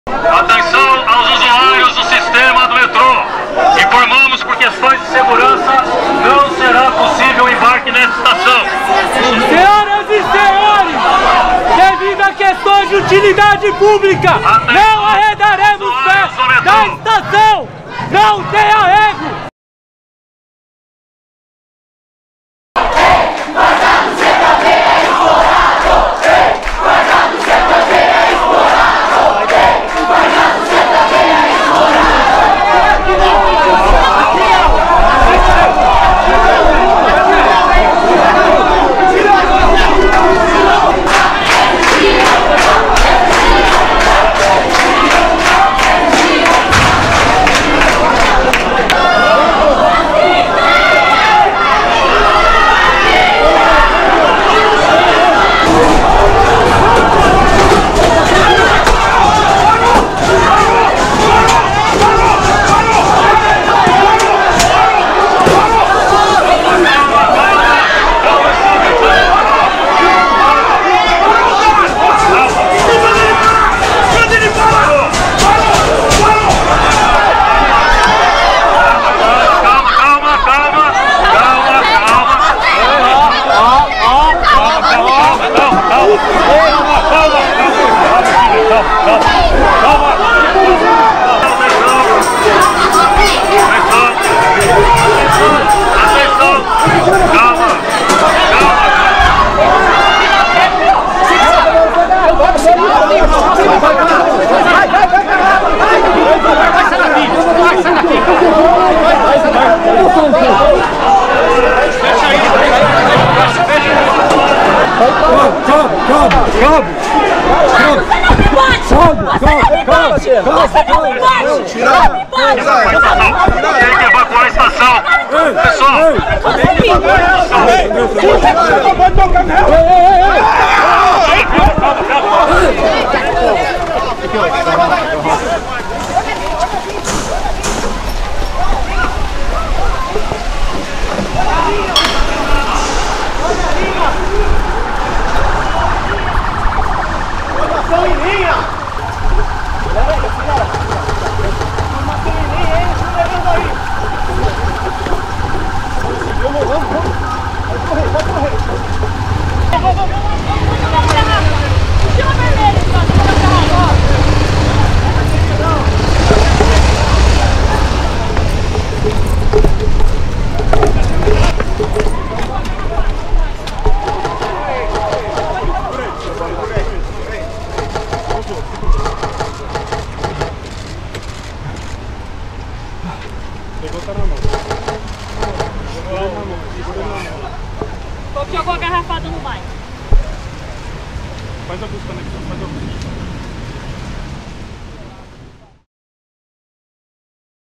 PM reprime manifestação do MPL no metrô em SP contra aumento da passagem - Ponte Jornalismo
A partir de R$ 5 por mês, você ajuda a construir a Ponte

As 32 pessoas detidas "para averiguação", inclusive um fotojornalista, foram liberadas nesta madrugada; 3 manifestantes foram autuados por desacato e dano.

O primeiro ato contra o novo aumento da tarifa de transporte público terminou com 32 pessoas detidas por volta das 22h, na última terça-feira (7/1) na cidade de São Paulo.

Só às 3h40 da madrugada da quarta-feira (8/1) os últimos ativistas foram liberados. Entre os detidos, segundo os advogados que acompanhavam o caso, 5 eram menores de idade. Eles foram encaminhados ao 78º DP (Jardins), onde 29 foram qualificados e 3 assinaram termo de circunstanciado de desacato e dano ao patrimônio.